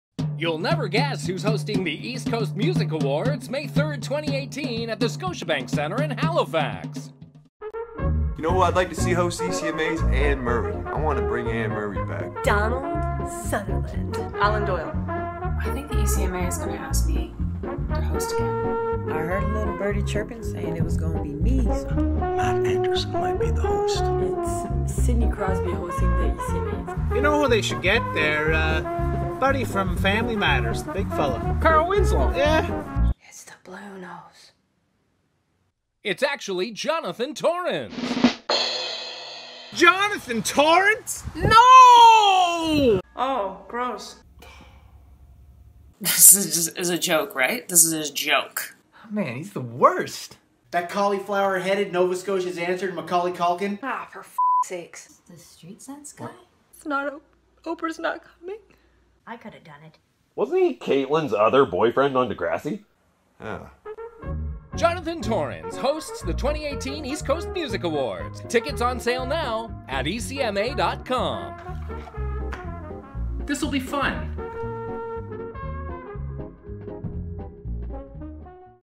Guess Who's Hosting The 2018 East Coast Music Awards Show?
The 2018 East Coast Music Awards: Festival & Conference return to Halifax, NS from May 2-6, 2018. The Awards Show takes place on May 3 at the Scotiabank Centre.